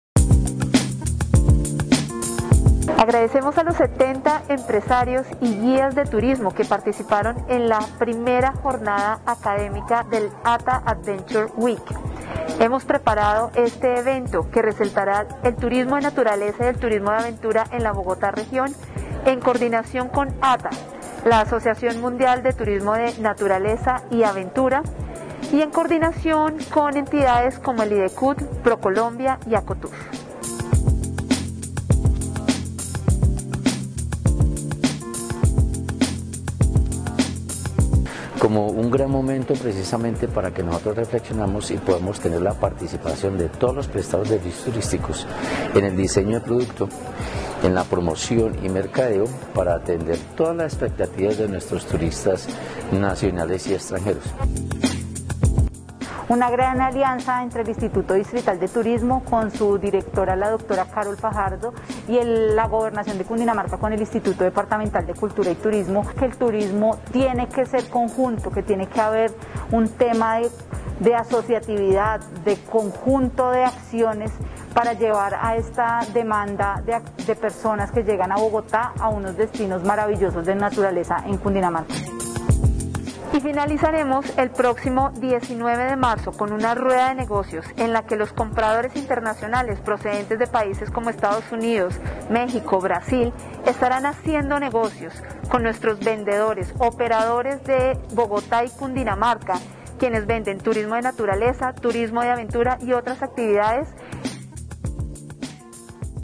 Karol Fajardo, directora del IDT habla sobre el Adventure Week
ComenzandoSemana |Nuestra directora @karol.fajardo_turismo, nos habla de la importancia del evento AwBogotá Adventure Week.